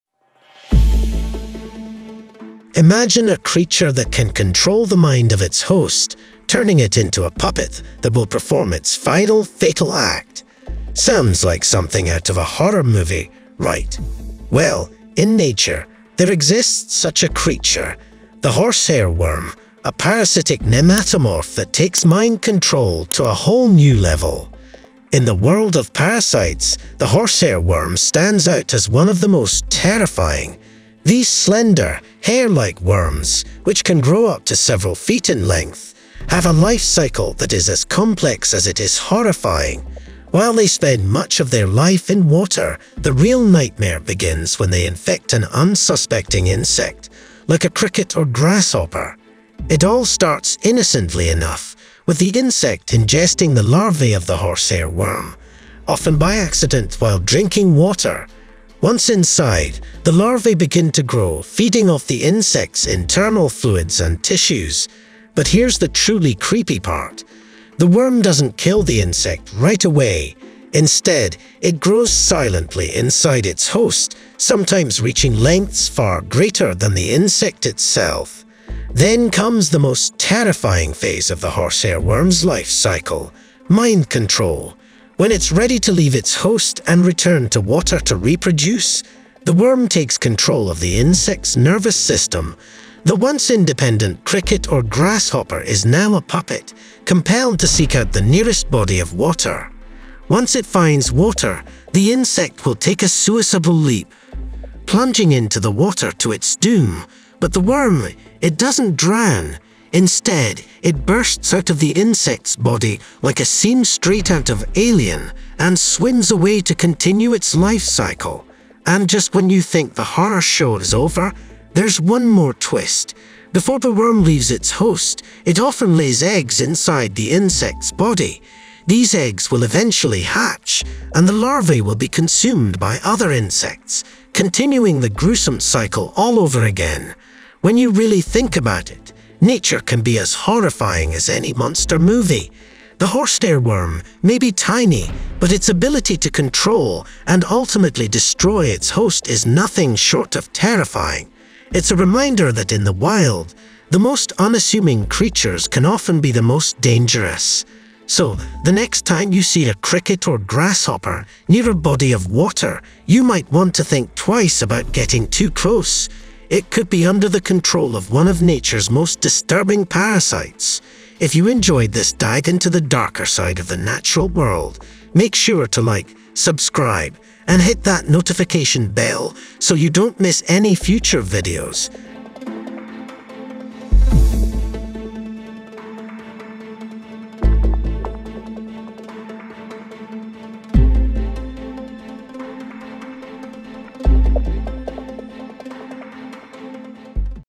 The Horsehair Worm - The Real Life Parasite That Creates Insect Zombies
Discover the terrifying truth about the Horsehair Worm, a parasitic creature that turns insects into mind-controlled zombies. Learn how this worm manipulates its host, forcing it to perform a fatal dive into water, where the worm gruesomely emerges to continue its life cycle. Nature's horror show is real, and it's more frightening than fiction!